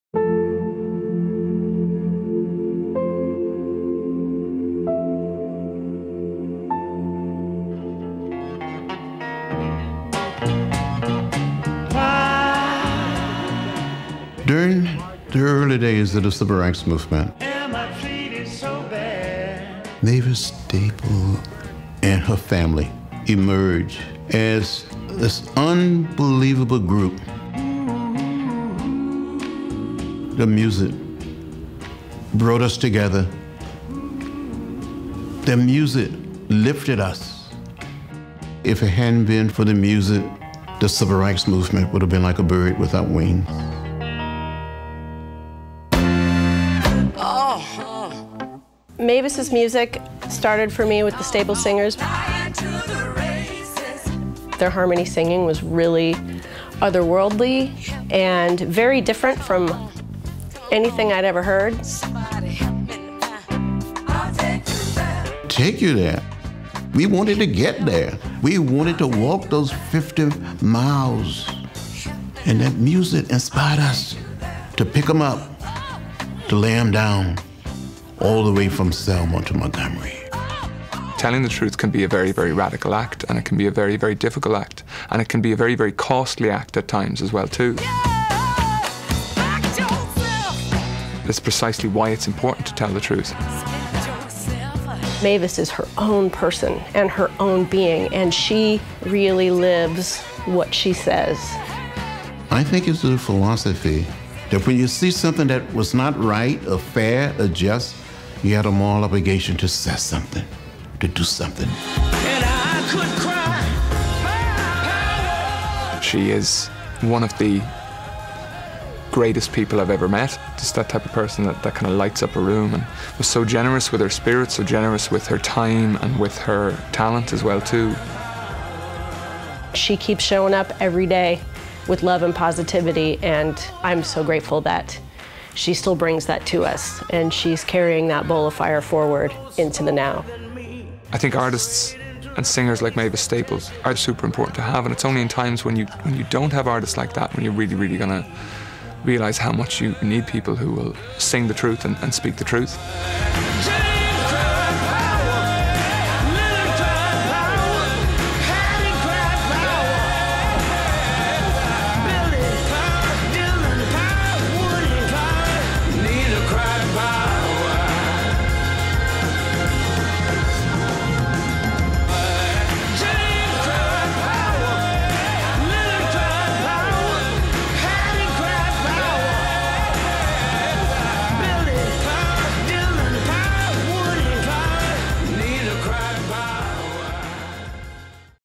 National Arts Awards 2018: Mavis Staples
Mavis Staples, music legend and civil rights activist, received the 2018 Carolyn Clark Powers Lifetime Achievement Award.